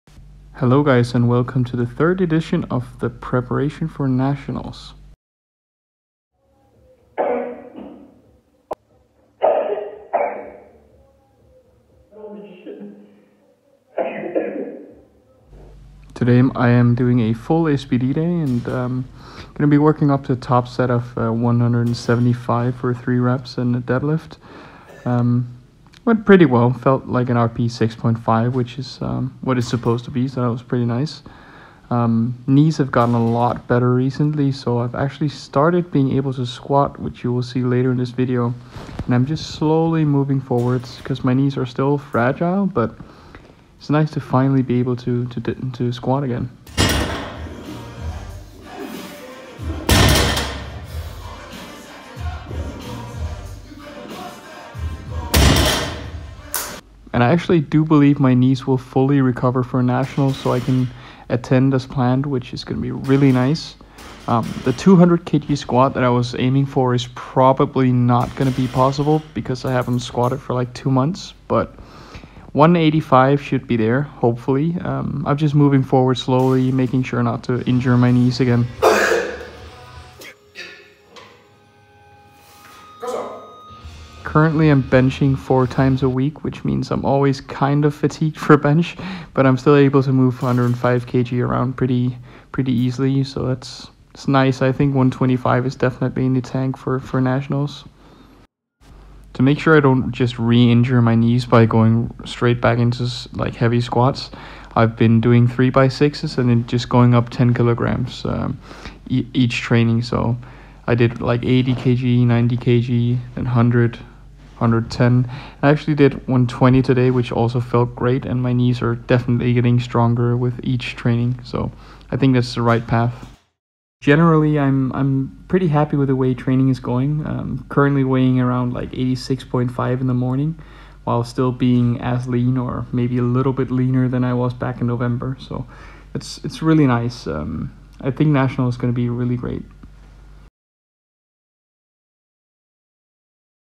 Prep for Nationals! • W9D1 - Knees improving : Testing STRONG smelling salts!
Was requested to make more of these , so i might start doing so:)